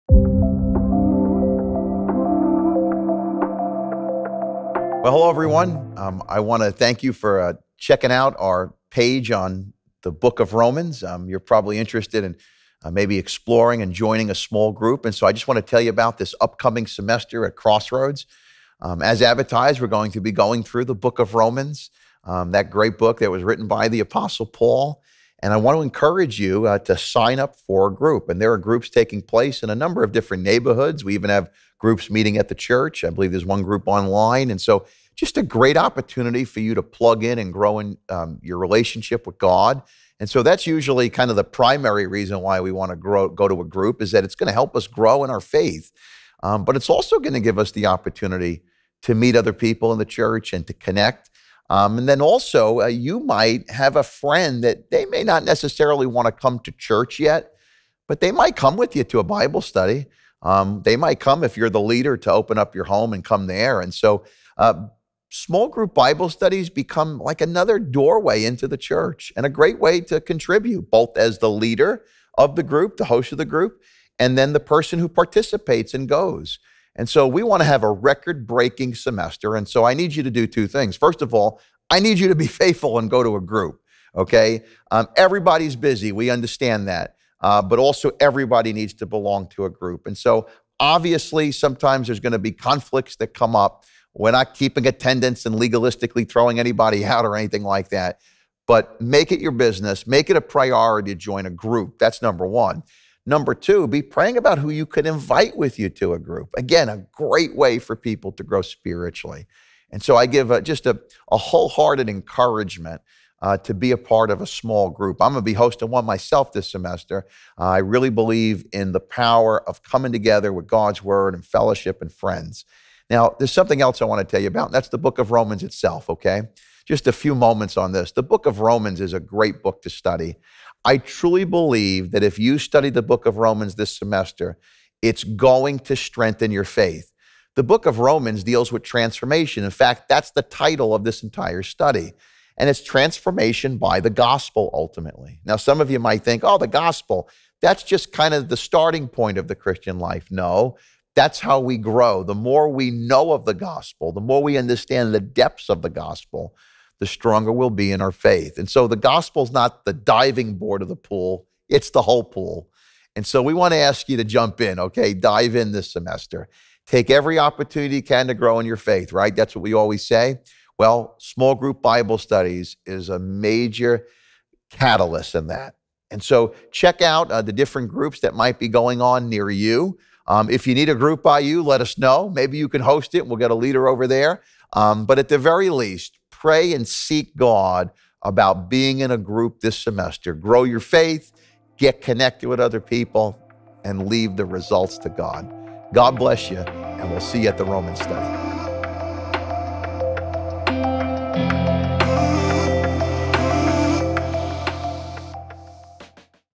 Welcome to Romans: Transformation Small Group!
Find Out More:
We are so excited for our new semester of Small Group. This semester is titled "Romans: Transformation" and we will be covering the book of Romans together.

We believe being part of a Small Group is a vital part of being in the church and in community with each other. We encourage everyone to join a group this semester.
Groups begin the week of September 16th.
For more information and to sign up